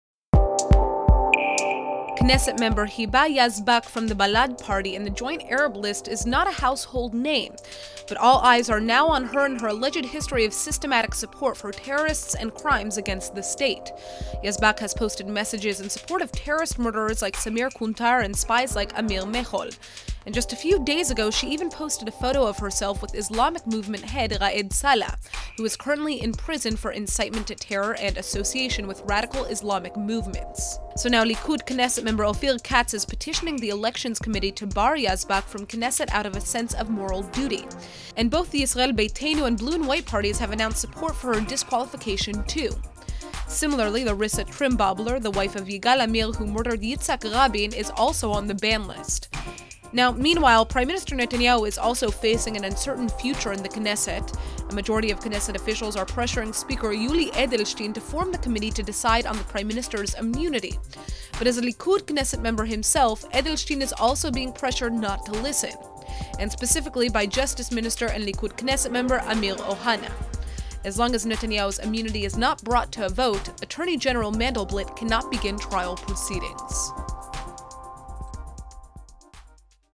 Arab MK supports terrorism?
An Arab-Israeli Knesset Member is now under the spotlight, for her consistent support for terrorism.